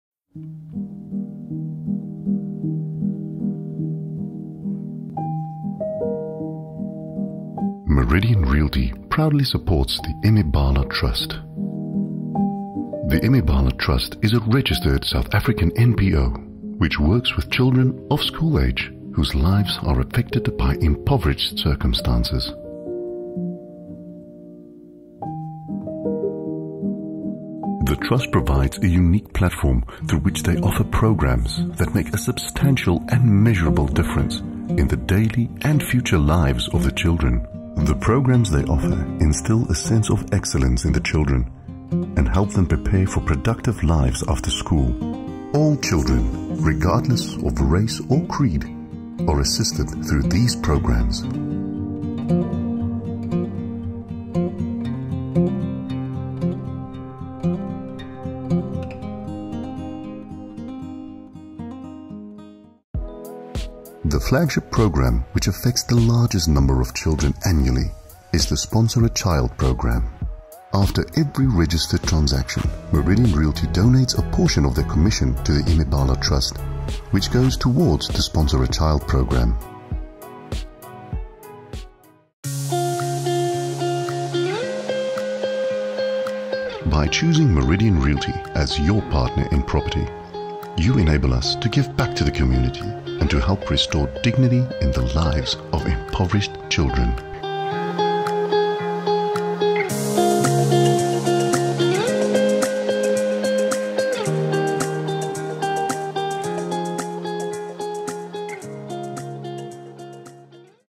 Meridian Realty joins forces with The Imibala Trust
The Imibala Trust is a registered South African NPO which works with children of school-age whose lives are affected by impoverished circumstances. The Trust provides a unique platform through which they offer programmes that make a substantial and measurable difference in the daily and future lives of the children. The programmes they offer instil a sense of excellence in the children and helps them prepare for productive lives after school. All children, regardless of race or creed, are assisted through these programmes.

The flagship programme which affects the largest number of children annually is the Sponsor-a-Child programme. The effects of persistent poverty continue to compromise children’s futures and hamper their dignity, safety and ability to thrive in the school system.

After every registered transaction Meridian Realty donates a portion of their commission to the Imibala Trust, which goes towards the Sponsor-a-Child programme. By choosing Meridian Realty as your partner in property you enable us to give back to the community and to help restore dignity in the lives of impoverished children.